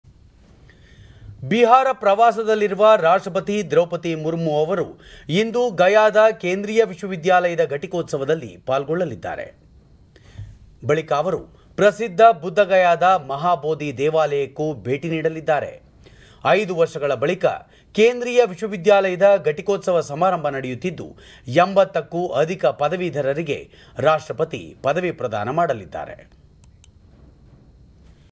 ಬಿಹಾರ ಪ್ರವಾಸದಲ್ಲಿ ರಾಷ್ಟ್ರಪತಿ ದ್ರೌಪದಿ ಮುರ್ಮು
ಬಿಹಾರ : ರಾಷ್ಟ್ರಪತಿ ದ್ರೌಪದಿ ಮುರ್ಮು ಗಯಾದಲ್ಲಿರುವ ದಕ್ಷಿಣ ಬಿಹಾರದ ಕೇಂದ್ರೀಯ ವಿಶ್ವವಿದ್ಯಾಲಯದಲ್ಲಿ ನಡೆಯಲಿರುವ ಘಟಿಕೋತ್ಸವ ಸಮಾರಂಭದಲ್ಲಿ ಭಾಗವಹಿಸಲಿದ್ದಾರೆ.